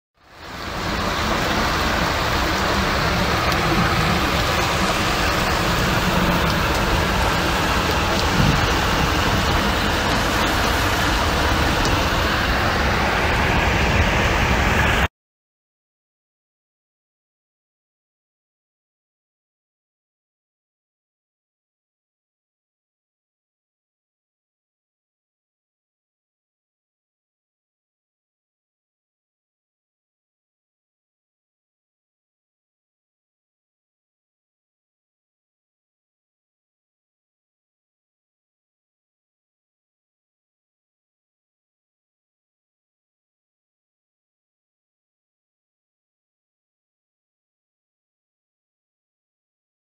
Cagliari, alluvione a Pirri
Cagliari, 10 settembre 2020 - Le strade di Pirri trasformate in fiumi in piena a causa della bomba d'acqua che si è abbattuta su Cagliari e il Sud Sardegna.